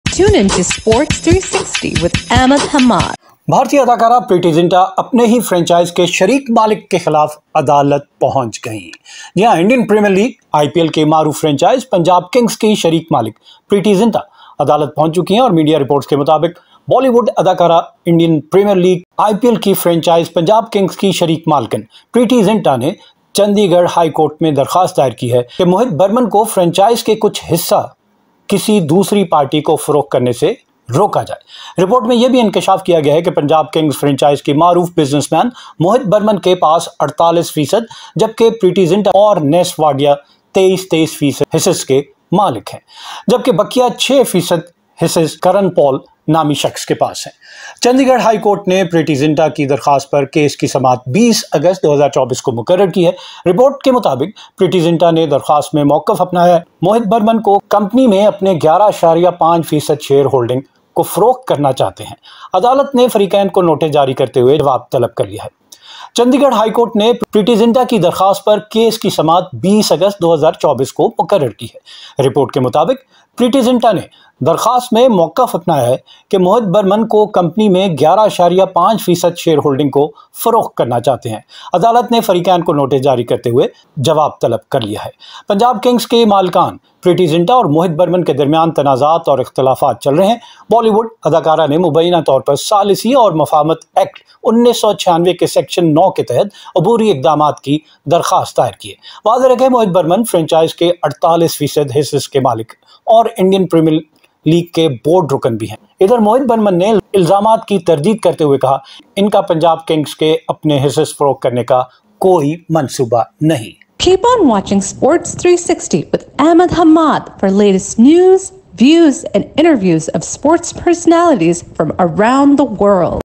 Preity Zinta Takes Legal Action Against Partner Over Stake Sale In Punjab Kings IPL Franchise.
Internal tensions are escalating within the ownership group of the Punjab Kings IPL franchise, with Preity Zinta, a 23% stakeholder through KPH Dream Cricket Private Limited, seeking a restraining order against co-owner Mohit Burman. Burman, who holds a 48% stake, is reportedly planning to sell part of his shares to Tomales Bay Capital L.P., a U.S.-based firm, which Burman denies. Zinta's legal action aims to prevent this sale, asserting that Burman did not offer the shares to existing promoters first, as required by internal agreements. The dispute, revealed during an April board meeting, involves a potential transaction valued around INR 540-600 crore, but Burman has priced it at $85 million. The case is set for a hearing on August 20, with the BCCI needing to approve any sale. The Punjab Kings, historically underperforming with only one final and one semifinal appearance in 17 years, is also seeking an Indian coach to replace Trevor Bayliss for the upcoming auction.